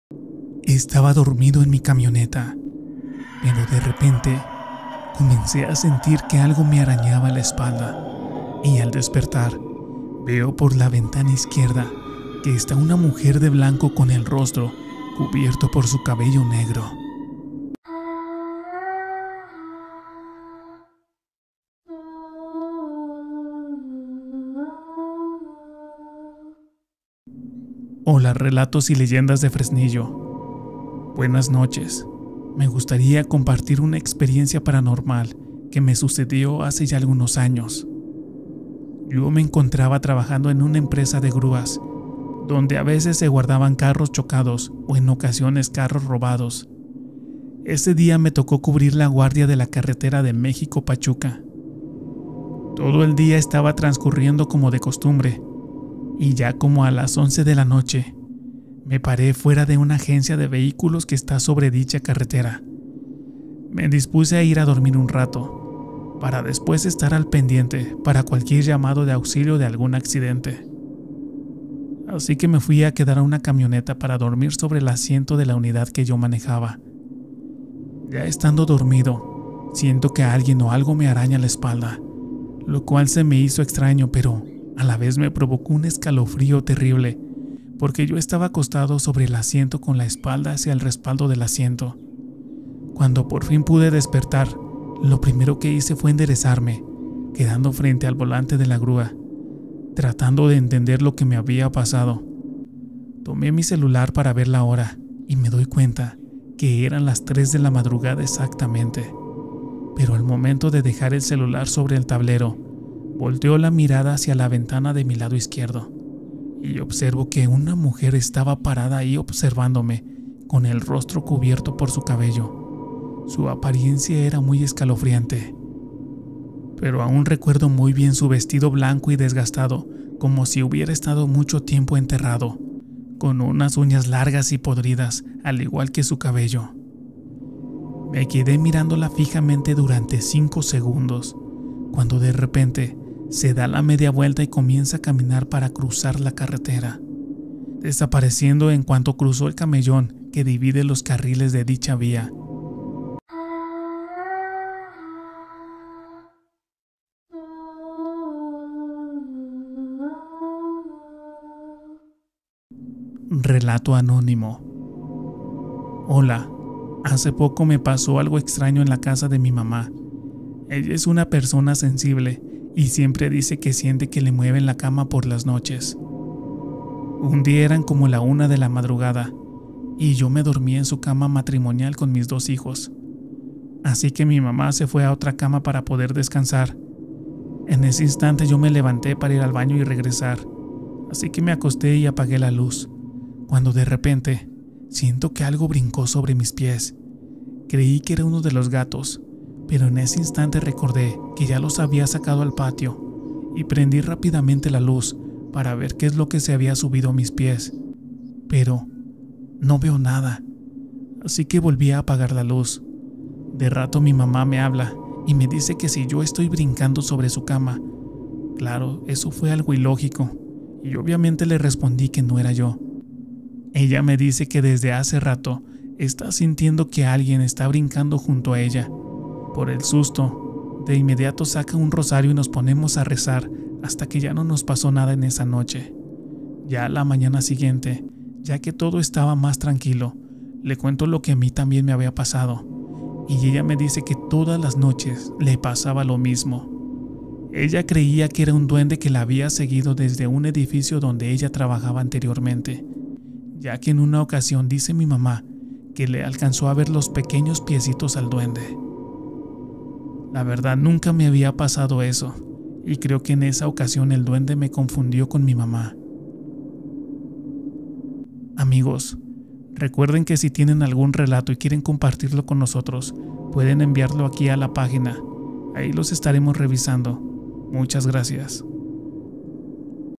2 historias de horror-Terror
Histrias de horror que valen la pena escuchar completas ya que en ellas se relata la historia escalofriante de un trabajador.